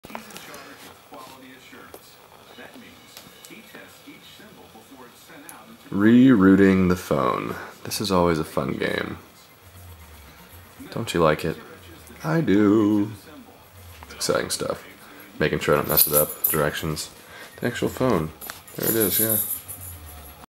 Stock Carrier Builds are for Babies
Rooting the Infuse to give "Refuse 1.2" a whirl.